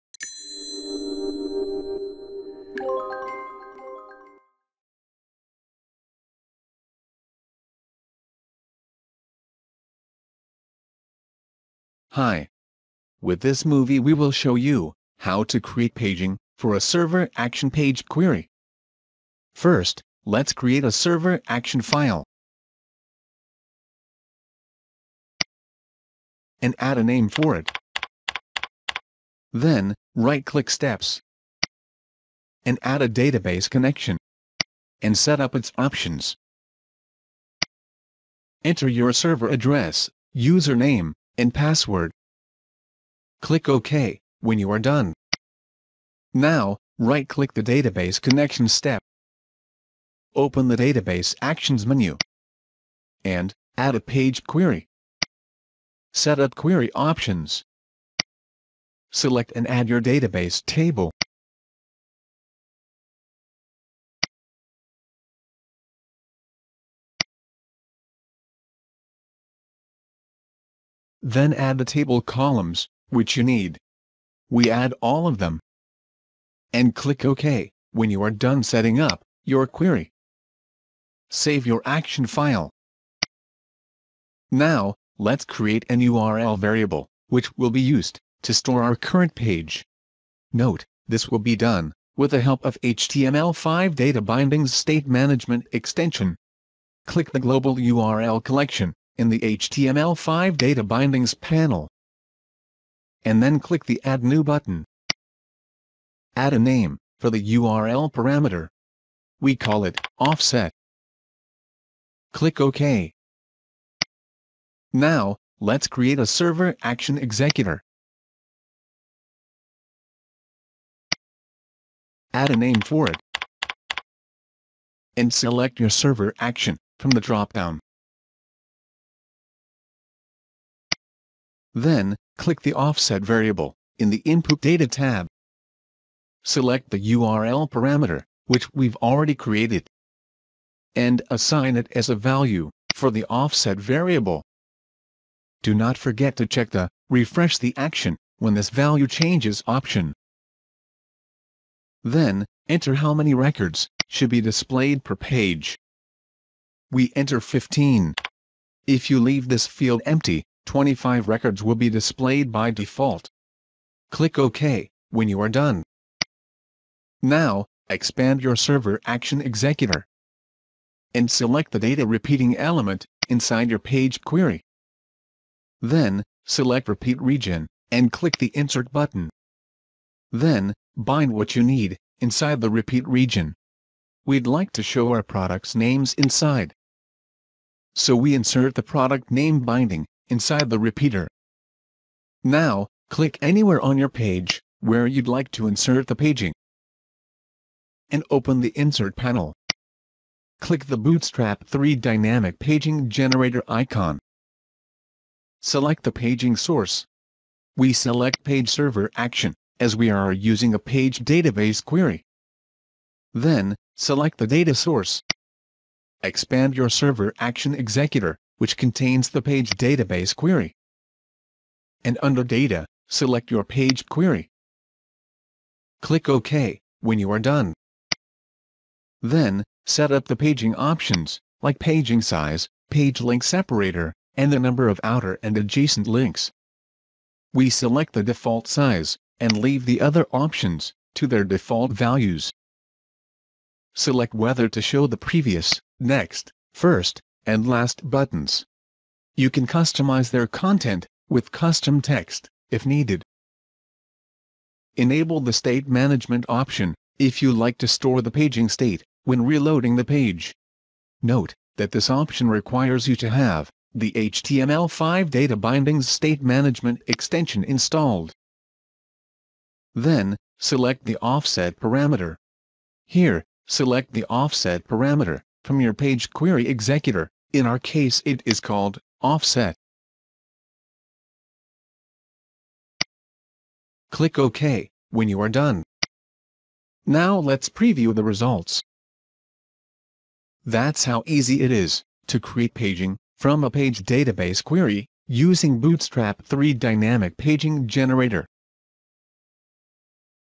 Creating Paging from Paged Database Query
With this movie we will show you how to create paging from a server action paged query.